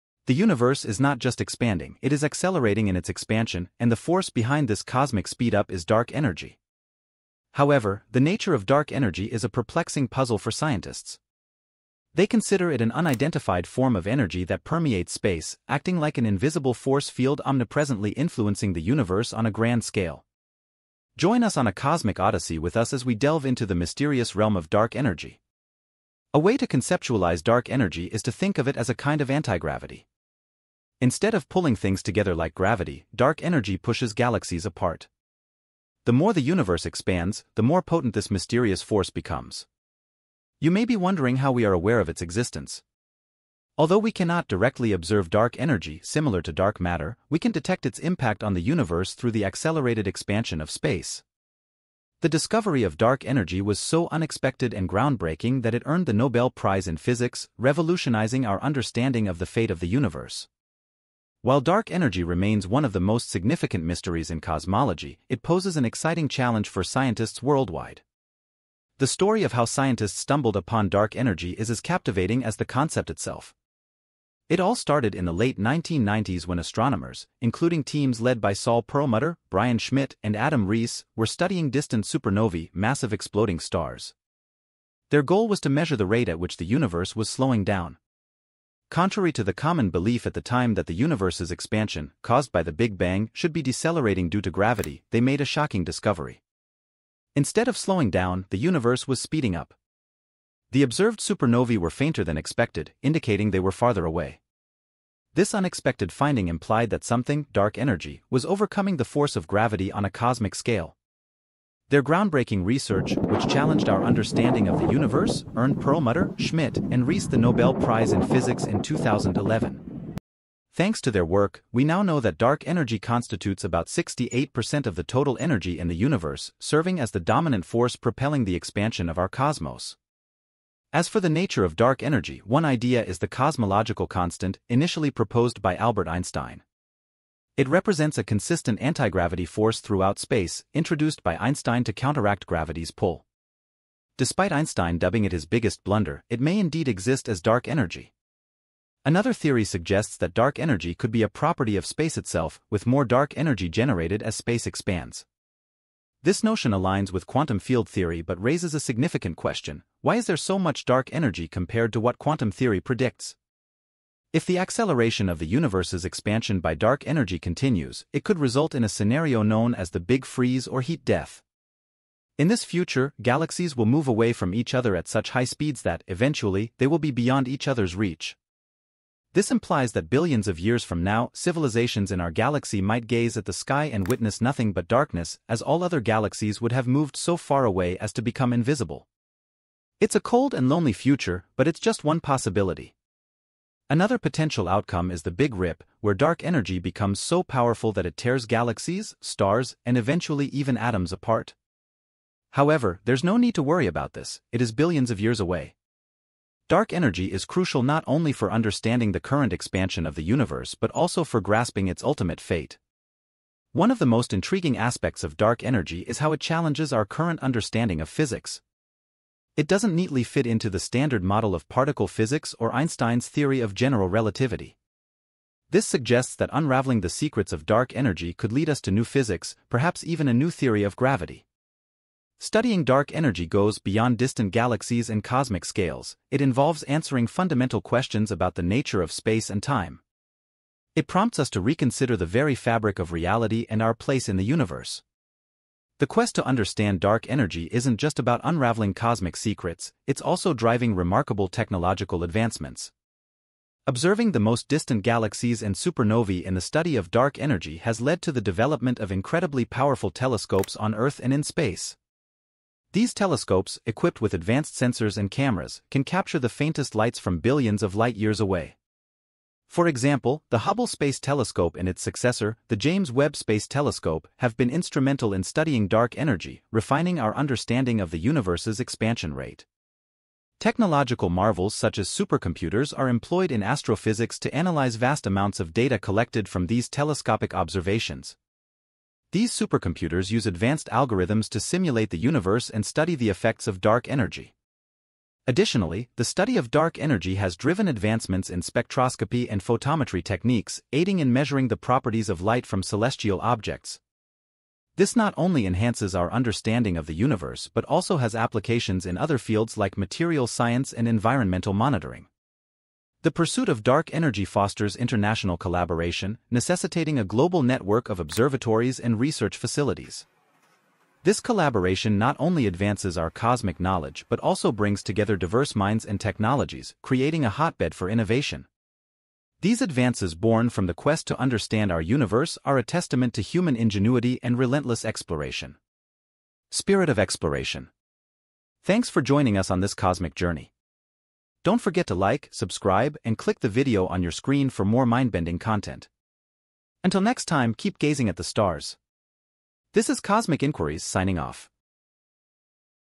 Unveiling the MYSTERY of DARK MATTER?
🔍 In this mind-bending exploration, we discuss the latest theories, groundbreaking research, and mind-boggling concepts surrounding Dark Energy. From its perplexing nature to its profound impact on the cosmos, we leave no cosmic stone unturned. 🌐✨